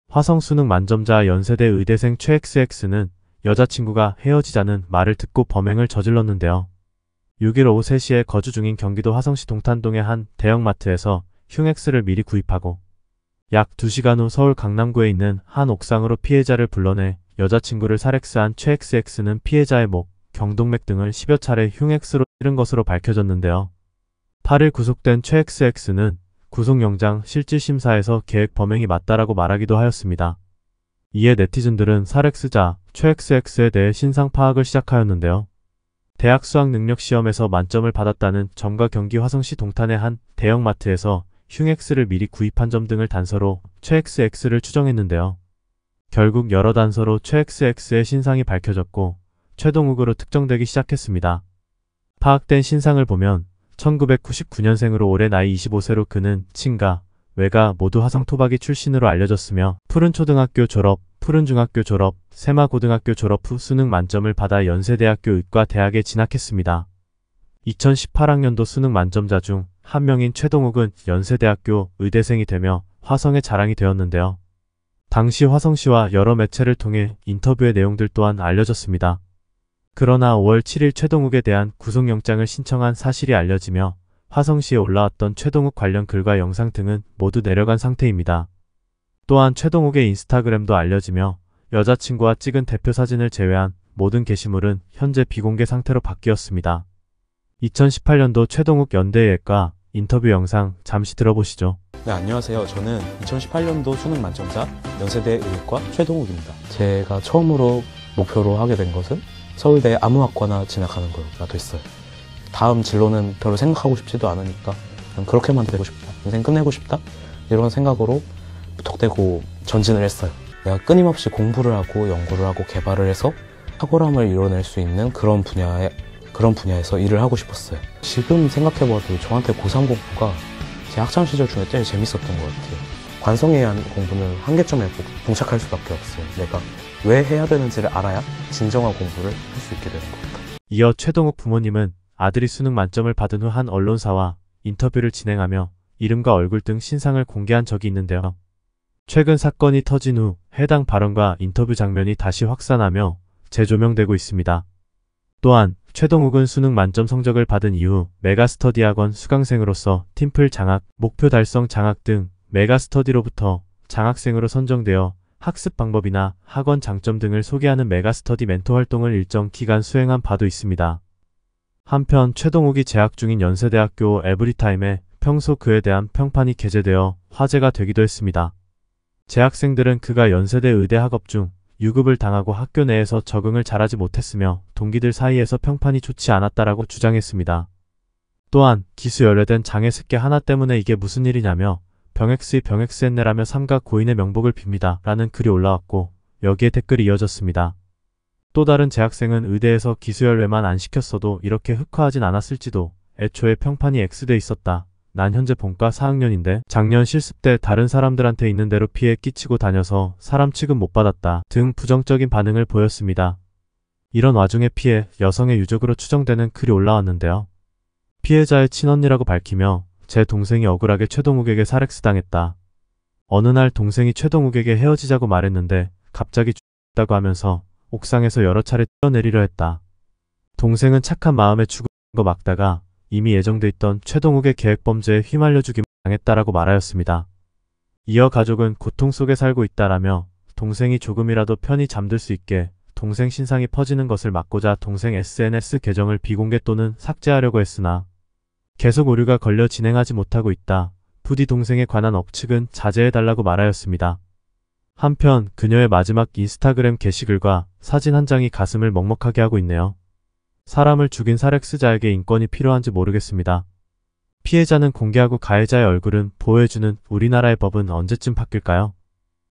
화성 수능 만점자 여친 살x 의대생 신상 털렸다
법좀 바꿔라...